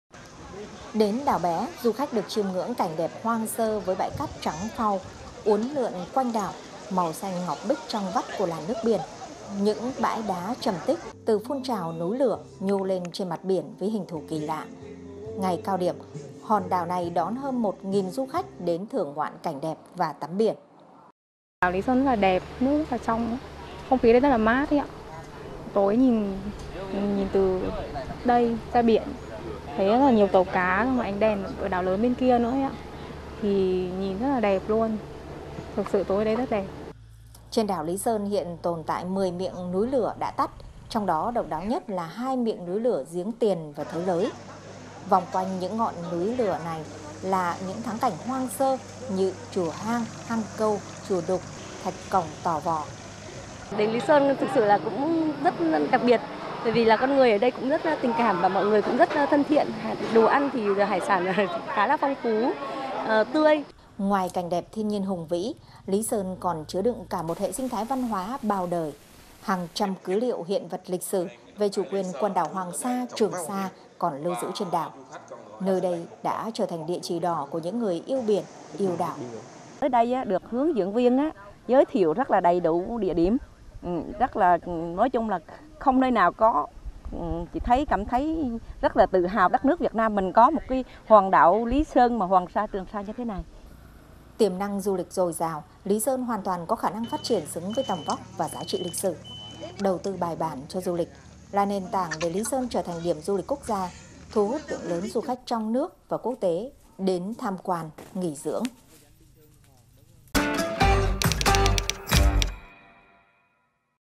DU LỊCH BIỂN ĐẢO LÝ SƠN HÚT KHÁCH | VTV5
Lý Sơn đón trên 73 ngàn du khách, tăng gần 34 ngàn so với cùng kỳ năm trước. Đảo Lý Sơn là điểm đến hấp dẫn và để lại ấn tượng đẹp trong lòng du khách vì giá trị địa chất, địa mạo, văn hóa, lịch sử.

Đón xem Bản tin Thời sự vào lúc 18h00 hàng ngày trên kênh VTV5 Tây Nguyên.